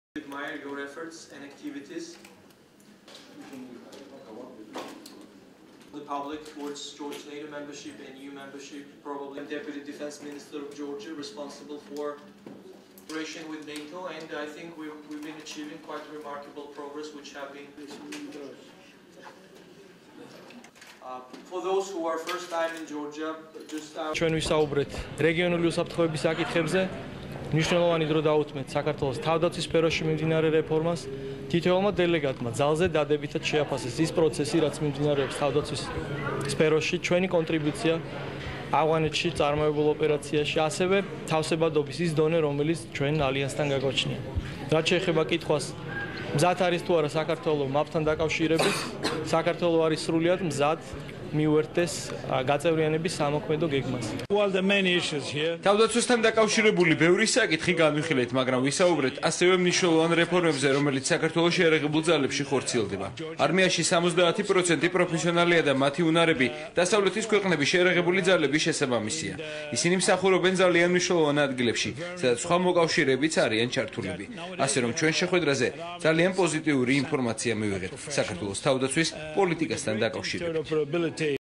შეხვედრა ნატოს საპარლამენტო ასამბლეის წარმომადგენლებთან
თავდაცვის მინისტრის მოადგილე მიხეილ დარჩიაშვილი ნატოს საპარლამენტო ასამბლეის პოლიტიკური კომიტეტის წარმომადგენლებს შეხვდა. შეხვედრა  დღეს „ქორთიარდ მარიოტში" გაიმართა.
მხარეებმა საქართველოს ნატოში ინტეგრაციის და სამომავლო პარტნიორობის საკითხები განიხილეს. საუბარი თავდაცვის სფეროში ნატო-სთან თავსებადობის კუთხით გატარებულ რეფორმებს და ამ მიმართულებით მიღწეულ პროგრესს შეეხო.
მიხეილ დარჩიაშვილმა კომიტეტის წევრებს თავდაცვის სამინისტროში განხორციელებული რეფორმები გააცნო და კიდევ  ერთხელ დაადასტურა, რომ ჩრდილოატლანტიკურ ალიანსში ინტეგრაცია საქართველოს ერთ-ერთი მთავარი პრიორიტეტია.
ნატოს საპარლამენტო ასამბლეის პოლიტიკური კომიტეტის წარმომადგენლებმა  თავდაცვის სფეროში გატარებული რეფორმები მოიწონეს და საქართველოს სრული მხარდაჭერა აღუთქვეს.
„თავდაცვასთან დაკავშირებული ბევრი საკითხი განვიხილეთ, მაგრამ ვისაუბრეთ ასევე მნიშვნელოვან რეფორმებზე, რომლებიც საქართველოს შეიარაღებულ ძალებში ხორციელდება. არმიში 70 პროცენტი პროფესიონალია და მათი უნარები დასავლეთის შეიარაღებული ძალების შესაბამისია. ისინი მსახურობენ ძალიან მნიშვნელოვან ადგილებში, სადაც სხვა მოკავშირეებიც არიან ჩართულები. ასე რომ, ჩვენ შეხვედრაზე ძალიან პოზიტიური ინფორმაცია მივიღეთ" - განაცხადა ნატოს საპარლამენტო ასამბლეის პოლიტიკური კომიტეტის თავმჯდომარემ ჟოზე ლელომ.
ნატოს საპარლამენტო ასამბლეის პოლიტიკური კომიტეტისა და საქართველო-ნატოს საპარლამენტთაშორისო საბჭოს წევრები შეხვედრებს დილიდან მართავენ. ისინი უკვე შეხვდნენ საქართველოს პარლამენტის წევრებს, პრეზიდენტს და საგარეო საქმეთა მინისტრის მოადგილეს. ხვალისთვის დაგეგმილია ვიზიტი ვაზიანის სამხედრო ბაზაზე.